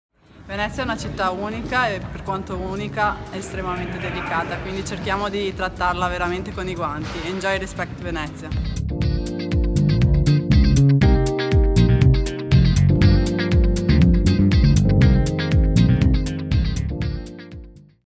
EnjoyRespectVenezia | Federica Pellegrini
La campionessa di nuoto Federica Pellegrini, testimonial della campagna internazionale di tutela e decoro di